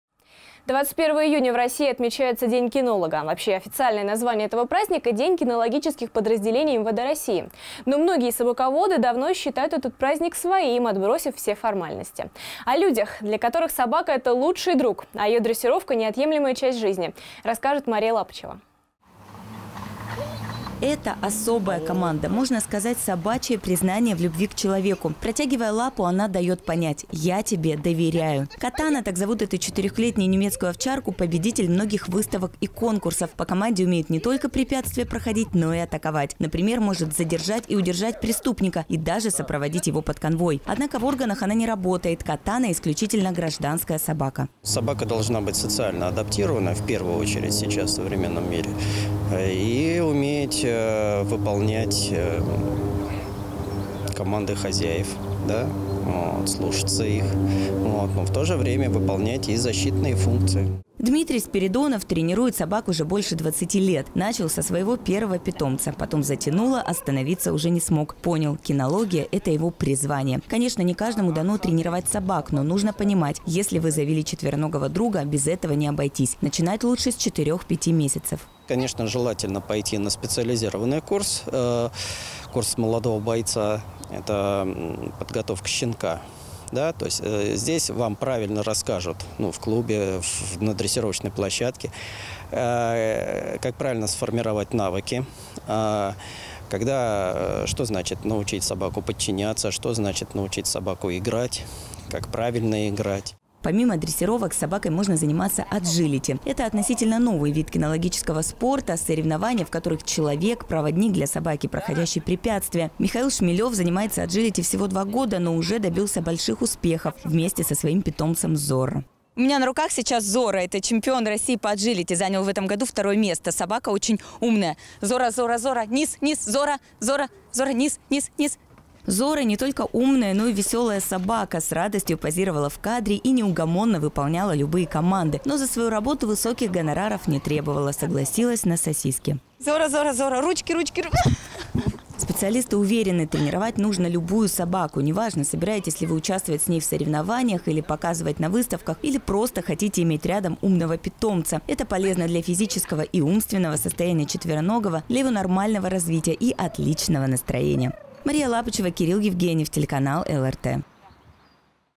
21 июня – День кинолога
21 июня в России отмечается день кинолога. Вообще, официальное название этого праздника – день кинологических подразделений МВД России. Но многие собаководы давно считают этот праздник своим, отбросив все формальности. О людях, для которых собака – это не просто лучший друг, а неотъемлемая часть жизни, расскажет Мария Лапычева.

Это – особая команда. Можно сказать, собачье признание в любви к человеку. Протягивая лапу, она дает понять: я тебе доверяю. Катана – так зовут эту 4х-летнюю немецкую овчарку – победитель многих выставок и конкурсов. По команде умеет не только препятствия проходить, но и атаковать. Например, может задержать и удержать преступника и даже сопроводить его под конвой. Однако в органах она не работает. Катана – исключительно гражданская собака.

Дмитрий Спиридонов, кинолог, старший инструктор кинологического центра, г. Люберцы:
"Собака должна быть социально адаптирована, в первую очередь, сейчас, в современном мире. И уметь выполнять команды хозяев, слушаться их. Но в то же время выполнять и защитные функции"

Дмитрий Спиридонов тренирует собак уже больше 20 лет. Начал со своего первого питомца, потом затянуло, остановиться уже не смог. Понял: кинология – это его призвание. Конечно, не каждому дано тренировать собак. Но нужно помнить: если вы завели четвероногого друга, без этого не обойтись. Начинать лучше с 4 – 5 месяцев.

"Тут желательно пойти на специализированный курс – курс молодого бойца, это подготовка щенка. Здесь, в клубе, на тренировочной площадке, вам расскажут, как правильно сформировать навыки: что значит – научить собаку подчиняться, что значит – научить собаку играть, как правильно играть".

Помимо дрессировок, с собакой можно заниматься аджилити. Это – относительно новый вид кинологического спорта; соревнования, в которых человек – проводник для собаки, проходящей препятствия. Михаил Шмелев занимается аджилити всего два года, но уже добился больших успехов. Вместе со своим питомцем Зорро.

Мария Лапычева, корреспондент:
"У меня на руках сейчас – Зорро. Чемпион России по аджилити. Занял в этом году второе место. Собака очень умная".

Зорро – не только умная, но и веселая собака: с радостью позировала в кадре и неугомонно выполняла любые команды. Но за свою работу высоких гонораров не требовала, согласилась на сосиски.
Специалисты уверены: тренировать нужно любую собаку. Неважно, собираетесь ли вы участвовать с ней в соревнованиях или показывать на выставках, или просто хотите иметь рядом умного питомца. Это полезно для физического и умственного состояния четвероногого, для его нормального развития и отличного настроения.

Мария Лапычева, Кирилл Евгеньев